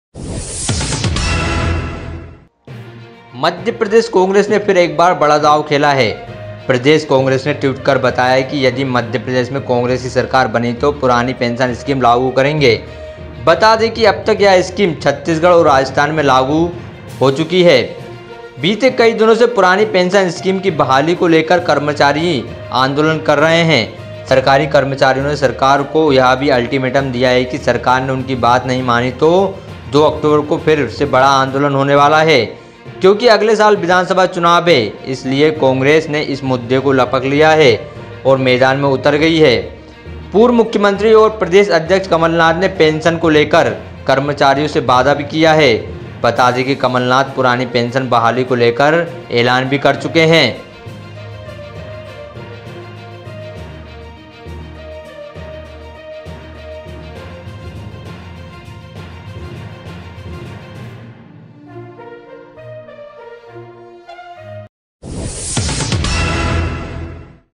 Congress के दांव से फंस गई BJP सरकार!
मध्य प्रदेश कांग्रेस ने फिर एक बार बड़ा दांव खेला है मध्य प्रदेश कांग्रेस ने ट्विट कर बताया की यदी मध्य प्रदेश में कांग्रेस की सरकार बनी तो पुरानी पेंशन स्कीम लागू करेंगे बता दें की अब तक यह स्कीम छत्तीसगढ़ और राजस्थान में कांग्रेस सरकारें यह स्कीम लागू कर चुकी है